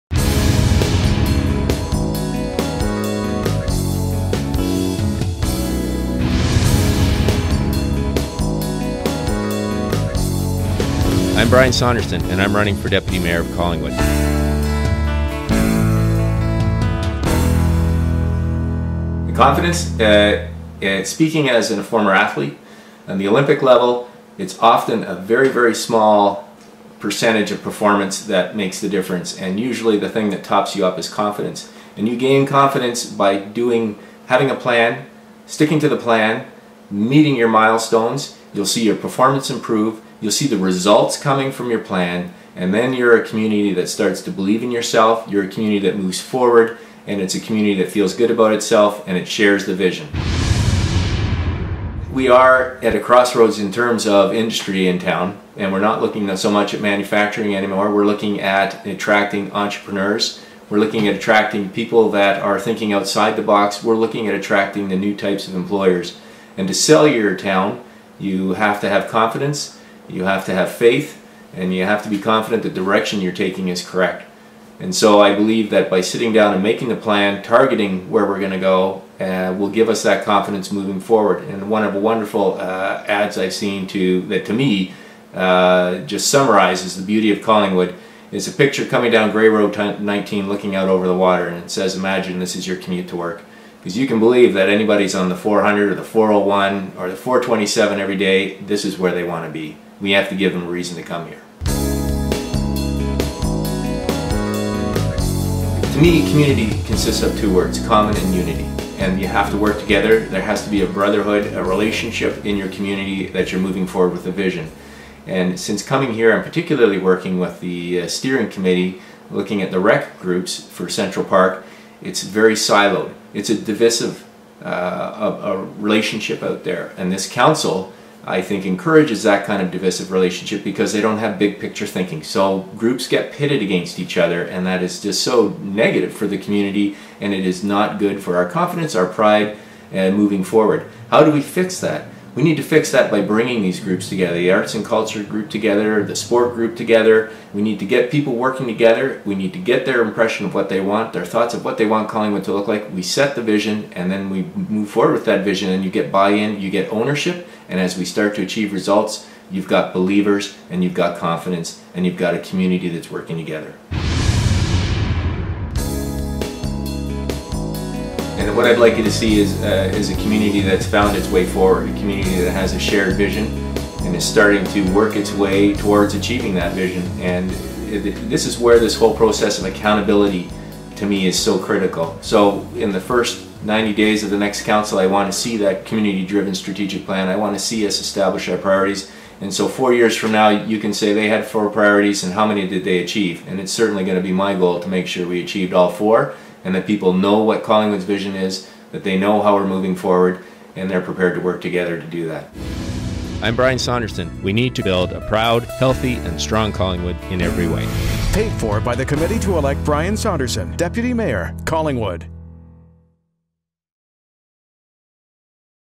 Brian Saunderson
Brian Saunderson wants to be Collingwood's next Deputy Mayor ... and he wants to see confidence return to the community.